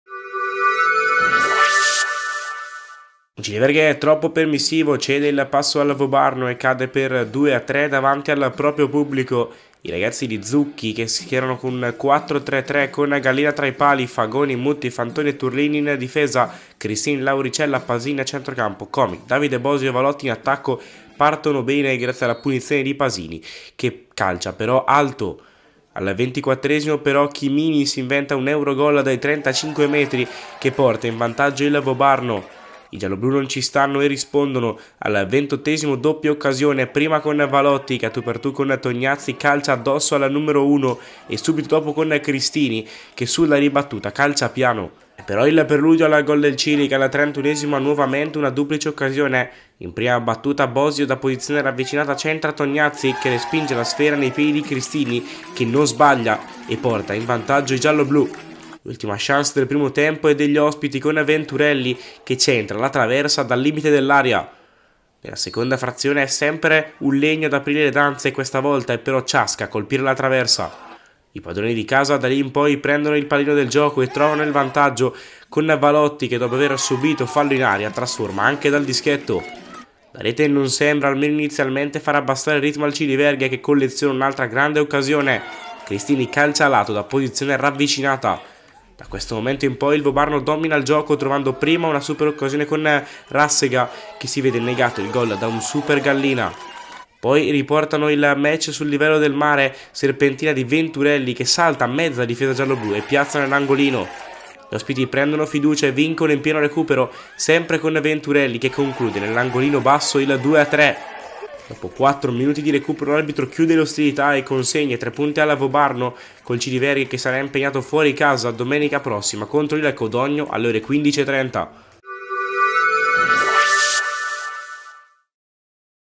HIGHLIGHTS| CILIVERGHE - VOBARNO
Dopo un primo tempo terminato sull’1-1 il Ciliverghe si porta avanti con Valotti ma si fa poi rimontare e superare dal Vobarno fino al 2-3 finale.